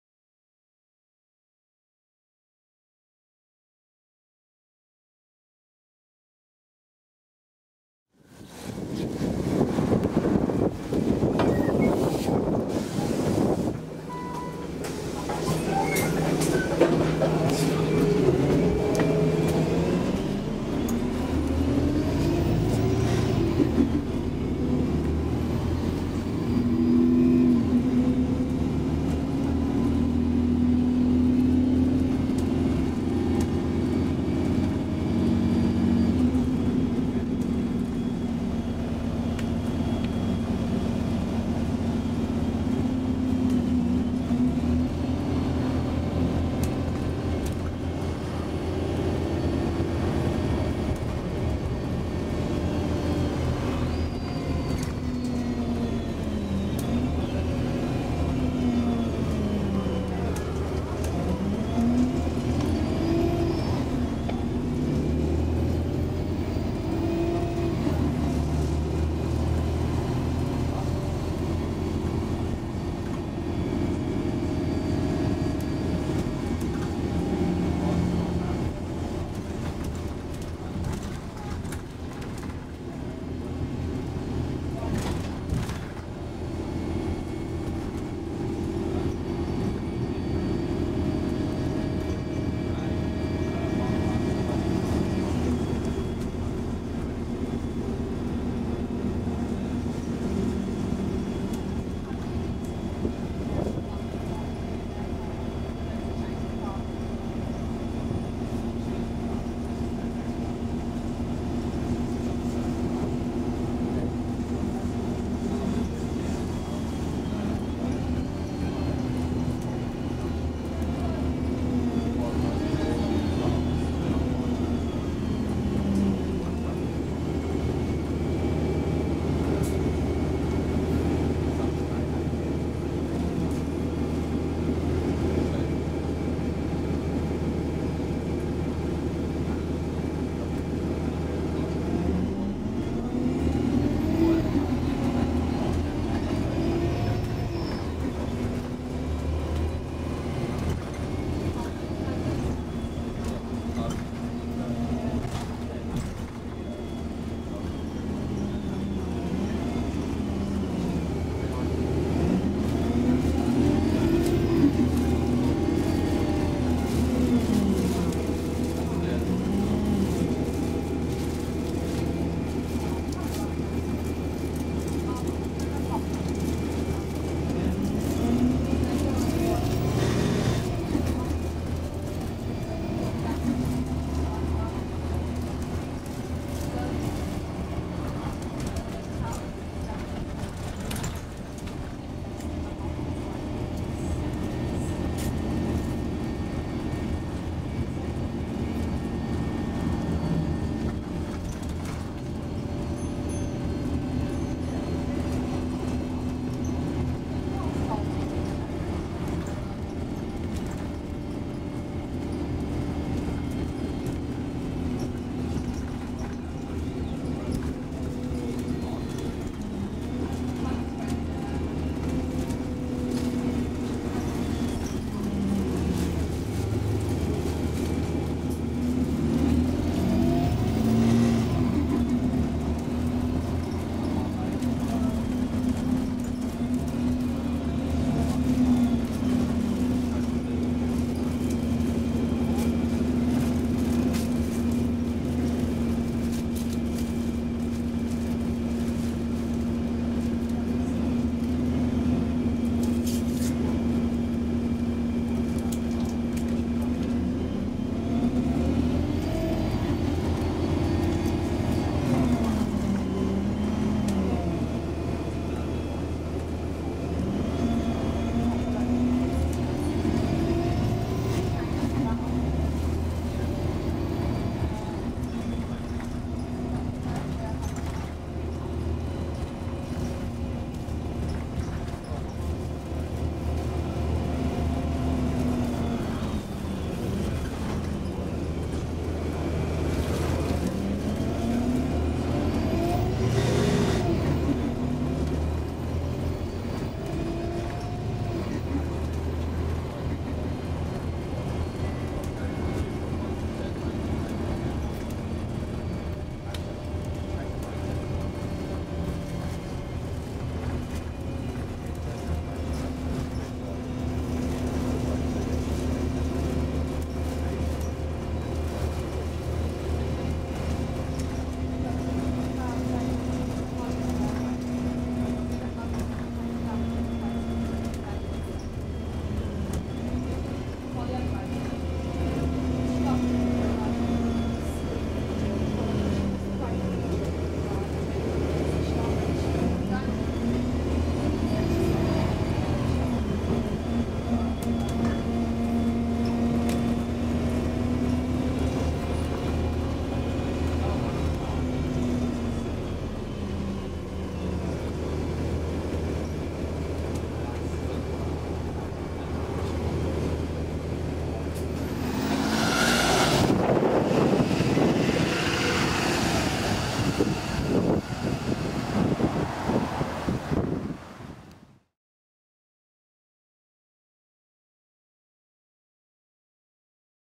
NX West Midlands | Alexander Dennis Enviro 200MMC | 2240 (YX65PXF) 'Liliana'
Operator - National Express West Midlands
Route - 71 Solihull - Sheldon - Tile Cross - Chelmsley Wood - Castle Vale - Minworth - Sutton Coldfield
Journey - 12:53 Solihull Station - Sutton Coldfield
Operating Garage - Acocks Green
Date Recorded - Saturday 20th August 2016
Vehicle Base - Acocks Green

Facebook Group - Rob's Public Transport Videos